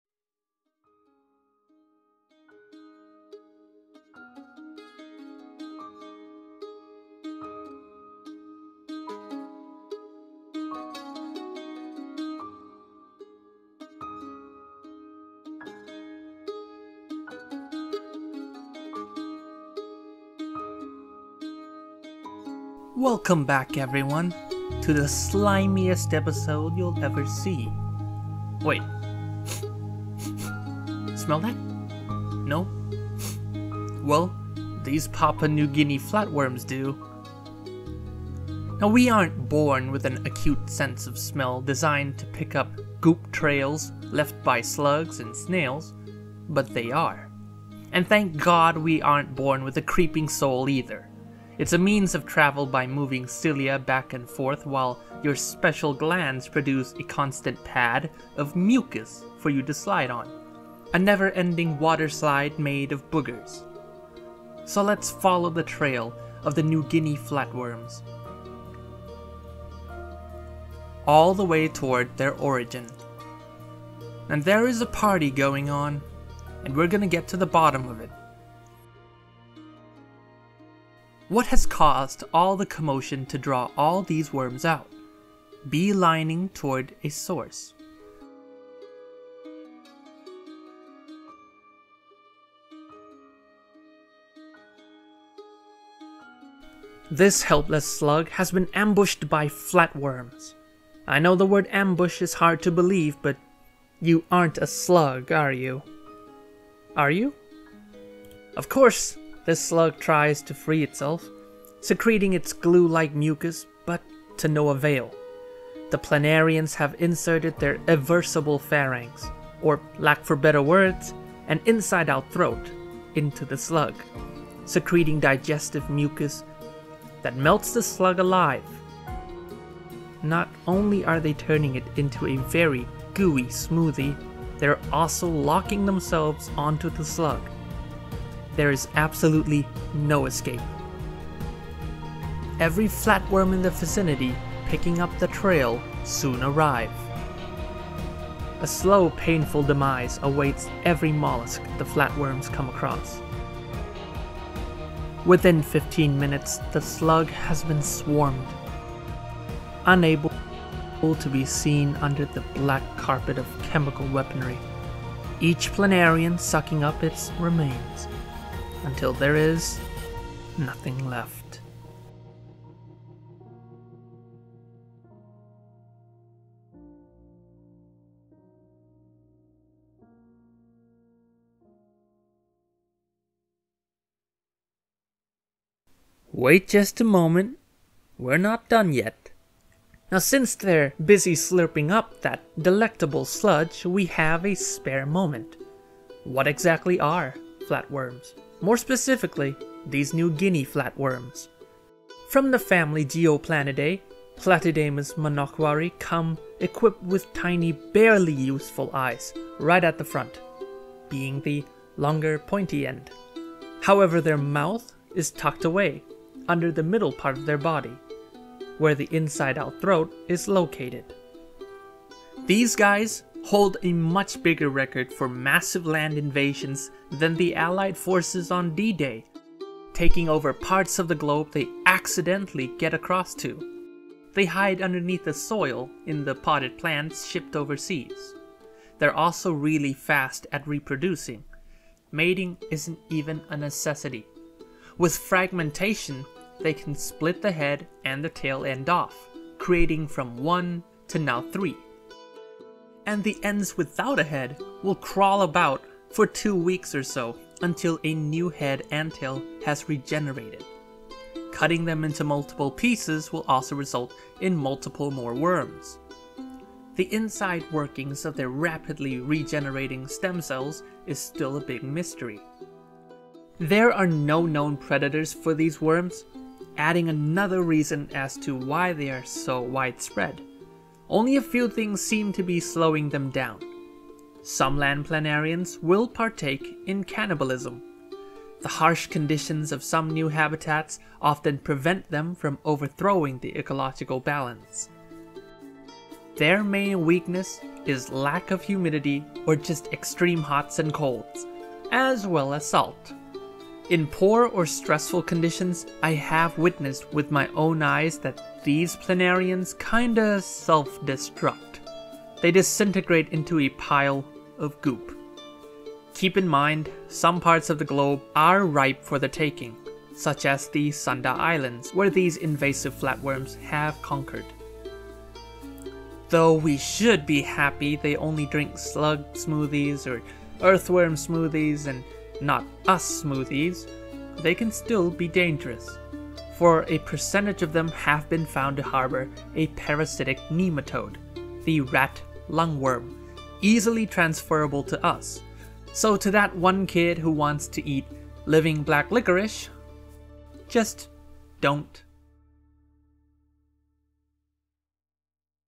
New Guinea Flatworms
Here I show you a glimpse into the life of an animal that the International union for the conservation of nature has dubbed. one of the 100 worst invasive species on the planet..

This video is to spread awareness on these mysterious invasive planarians.